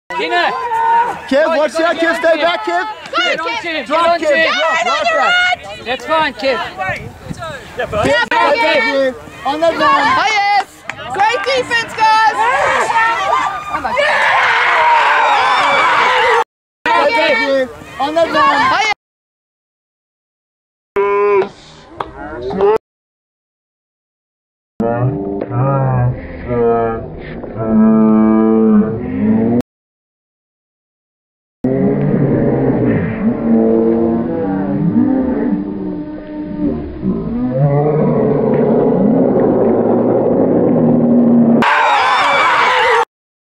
Worlds Greatest Ultimate Frisbee Play
It was called out due to Oliver stepping on the line. decide for yourself was it in or out?
Extreme Ultimate going on here...